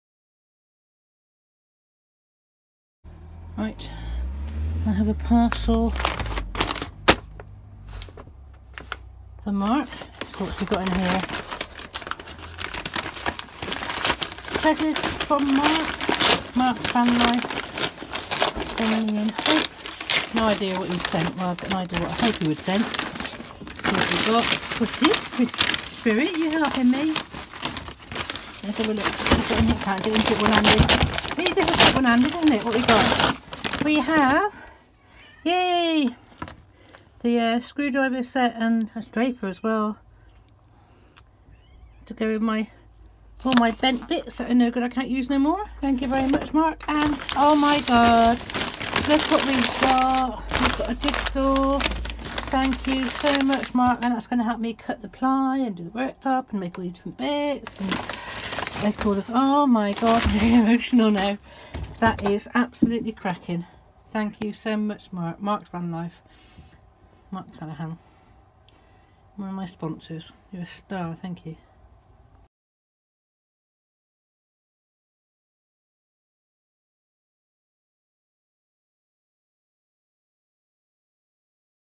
A surprise parcel .. Thankyou
A parcel from a sponsor to help me with rest of build!